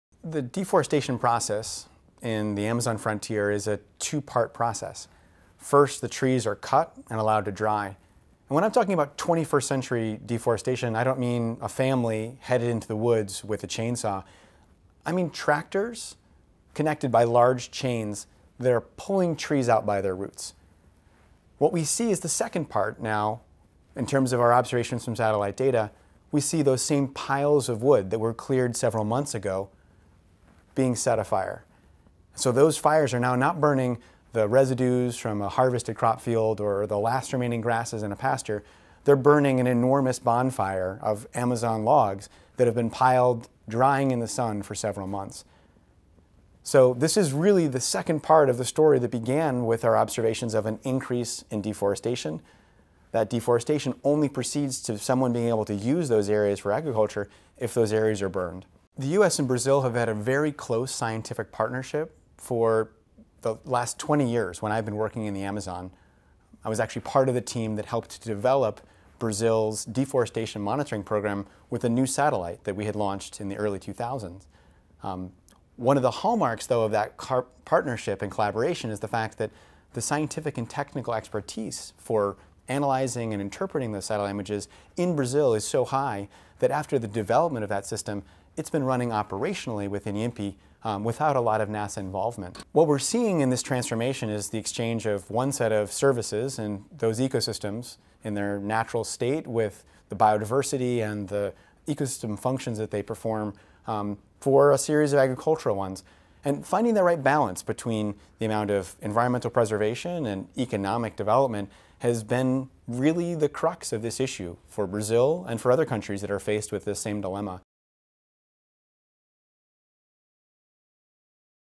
Cientista da Nasa analisa incêndios na Amazônia
Douglas Morton é um cientista da Nasa que participou da implantação do programa de monitoramento do desmatamento no Brasil. Para ele, o conhecimento científico e técnico para analisar e interpretar imagens de satélite no Brasil é tão avançado que, após a implantação conjunta do sistema de monitoramento, ele tem funcionado operacionalmente com o Inpe sem muito envolvimento da Nasa. No caso dos incêndios, ele alerta: "Não estão queimando os resíduos de um campo de cultivo depois da colheita, e sim pilhas de troncos que secaram ao sol por meses."